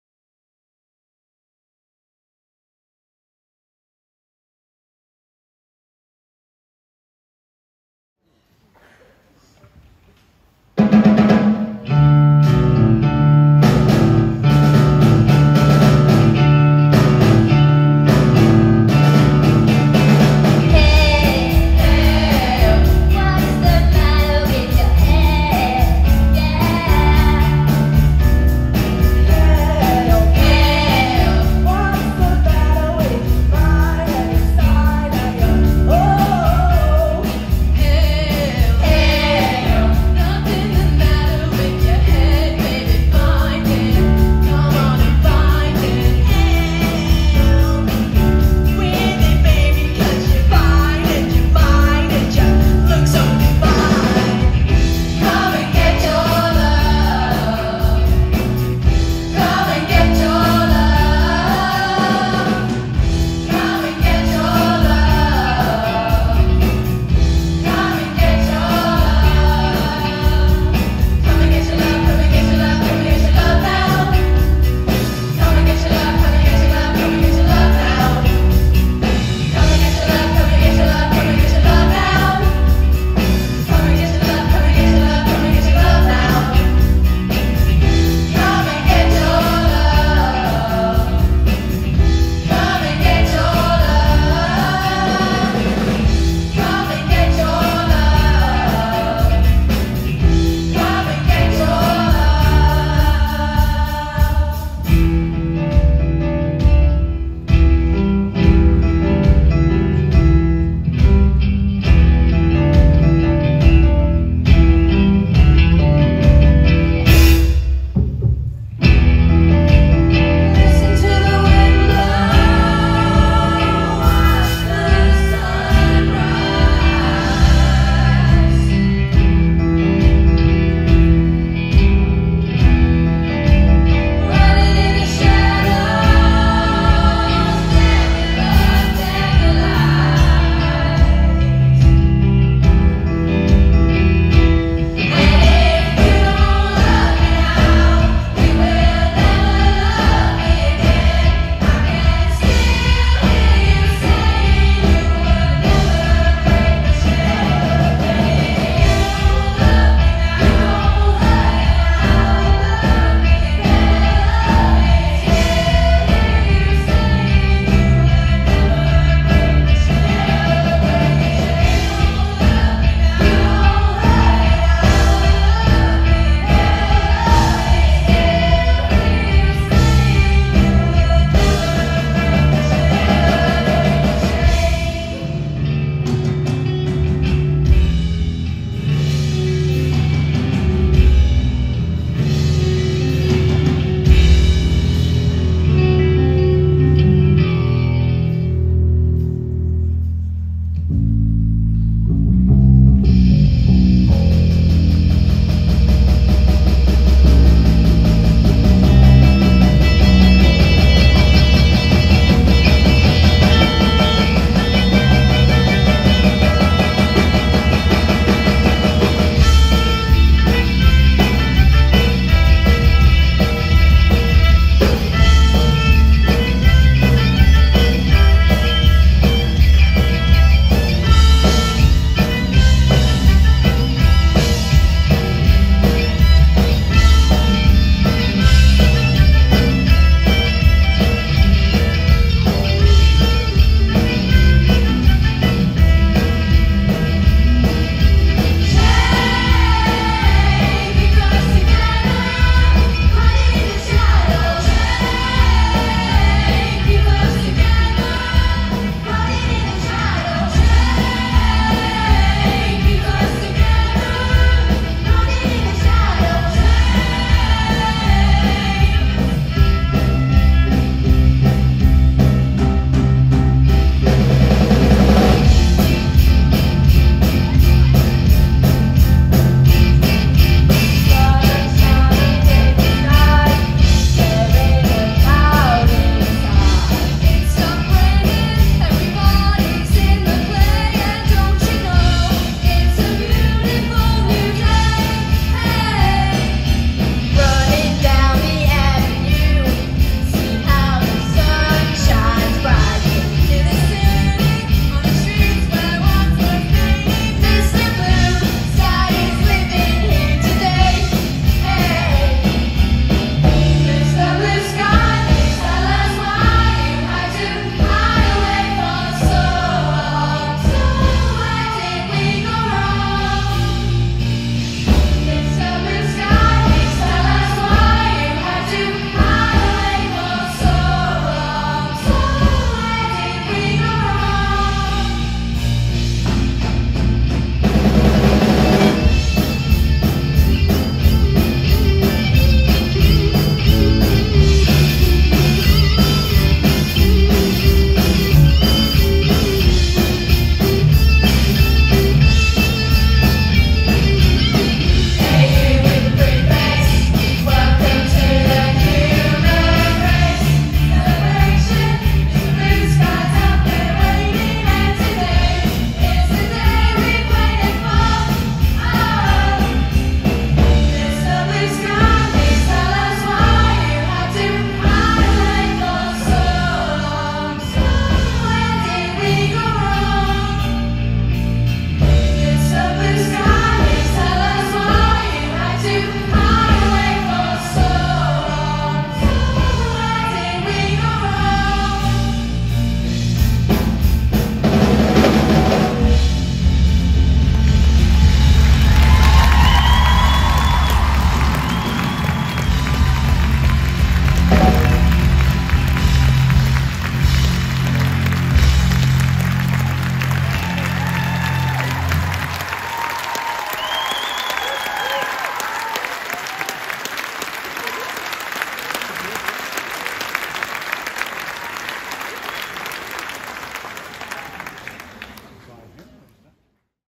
2024 NELT summer concert: A Night at the Movies
Relive our performance at the NELT summer concert at The Glasshouse, Gateshead.